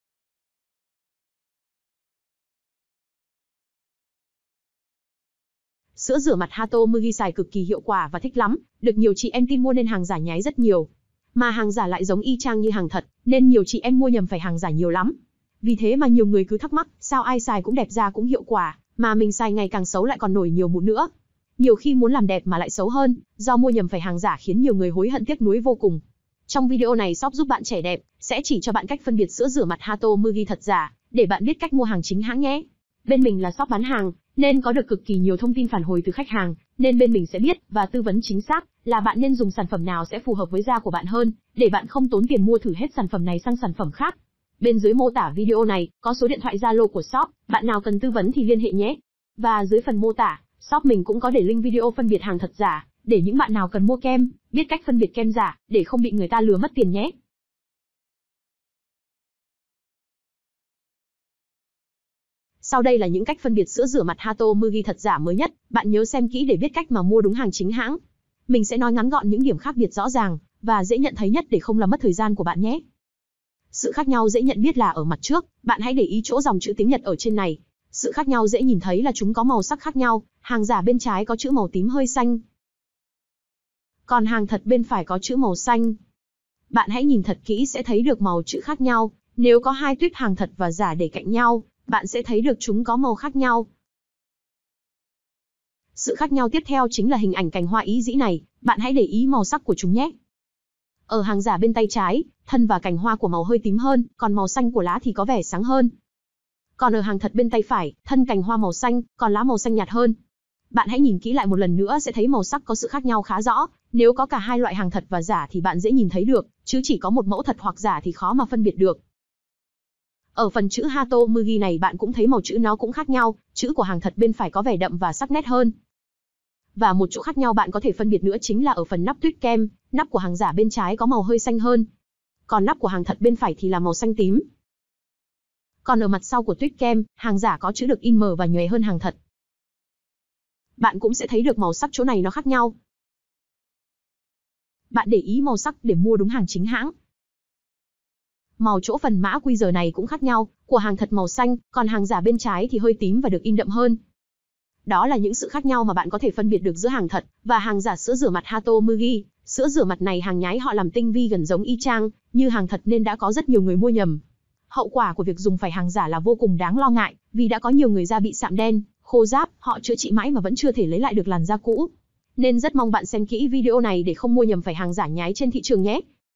Phân biệt sữa rửa mặt Hatomugi thật giả, SRM Ý Dĩ Fake của Nhật Bản
Video này, shop giúp bạn trẻ đẹp bên mình sẽ chỉ cho bạn cách nhận biết sữa rửa mặt Hatomugi hàng thật và giả khác nhau như thế nào nhé. Mong là bạn xem kỹ video để có thể nhận ra được hàng thật và hàng giả sữa rửa mặt Hatomugi có những sự khác nhau như thế nào.
Dù sao thì bạn cũng nên mua đúng hàng chính hãng để đảm bảo an toàn cho da mặt của bạn nhé.